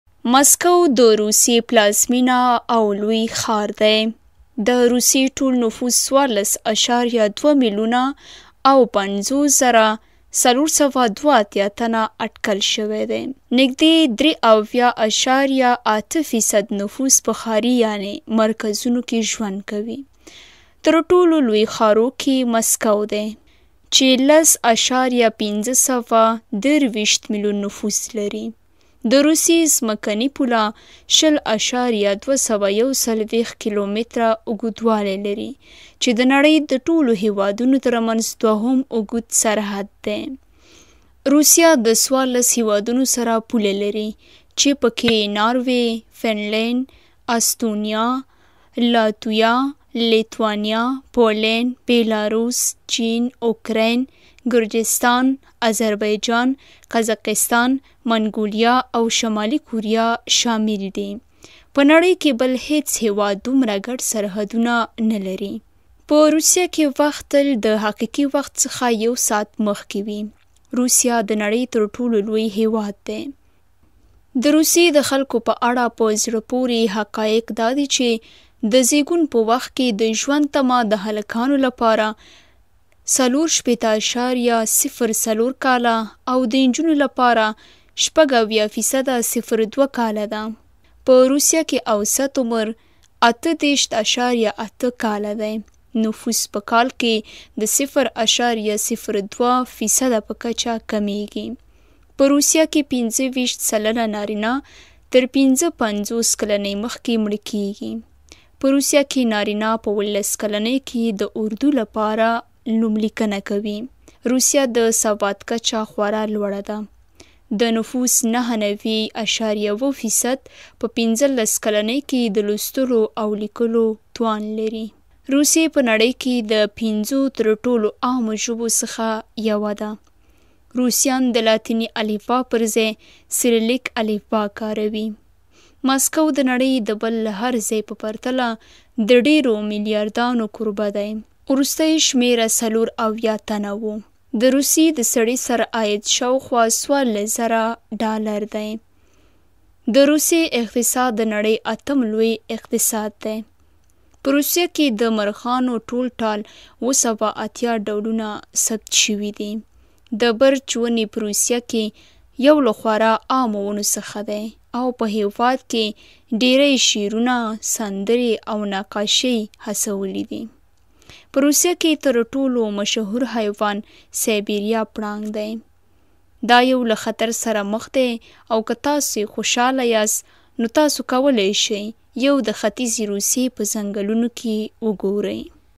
د روسیې په اړه په زړه پورې حقایق
روسیه د 14 هیوادونو سره پوله لري چې په کې ناروې، فنلینډ، استونیا، لاتویا، لیتوانیا، پولنډ، بیلاروس، چین، اوکراین، ګرجستان، اذربایجان، قزاقستان، منګولیا او شمالي کوریا شامل دي. په نړۍ کې بل هیڅ هیواد دومره ګډ سرحدونه نلري.

څرک میډیا
Srakmedia